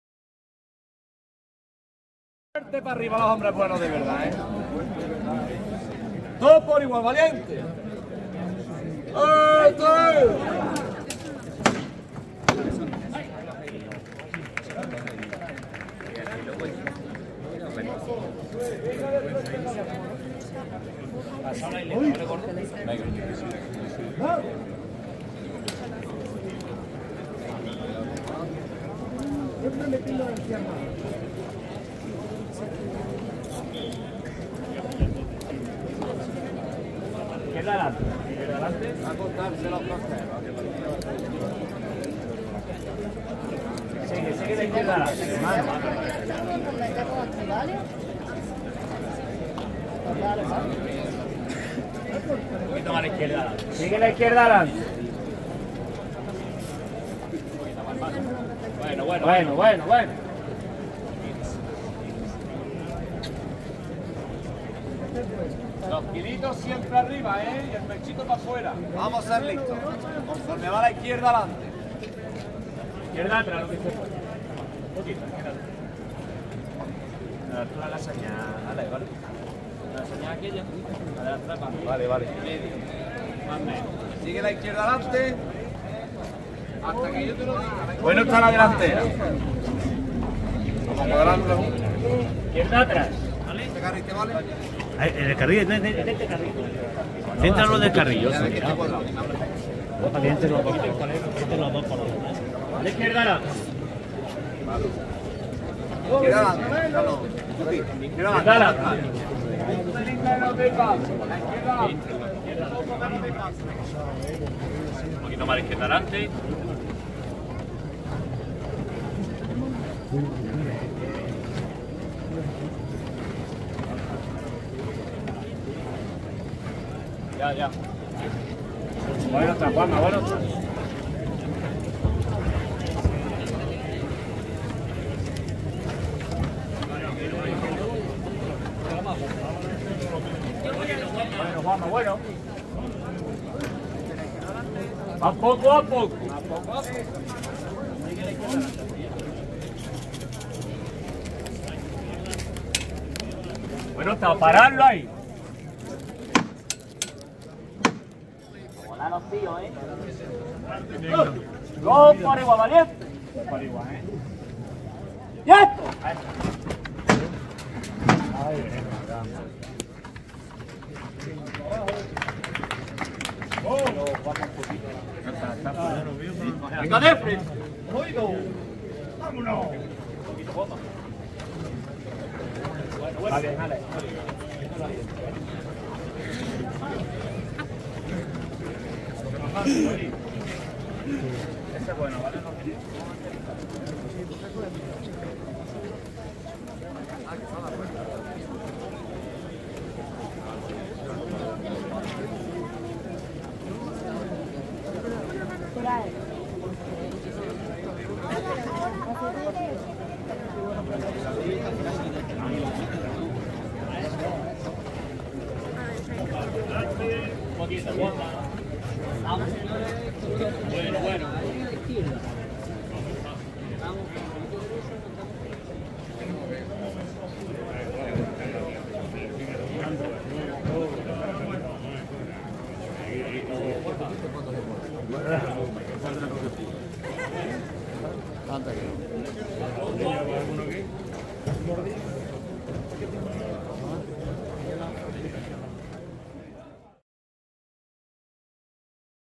El Cristo de la Redención camino de Catedral - Traslado Magna de Granada
Procesión Magna "La Pasión según Granada" celebrada en la jornada del 14 de octubre con motivo del 34º Encuentro Nacional de Cofradías.
Un total de 22 pasos procesionales salieron a la calle para conmemorar dicha efeméride.
En las jornadas previas se realizó el traslado de las imágenes en sus pasos procesionales hasta la Catedral granadina.

Santísimo Cristo de la Redención, titular de la hermandad de Redención y Salud. Avenida de Dílar.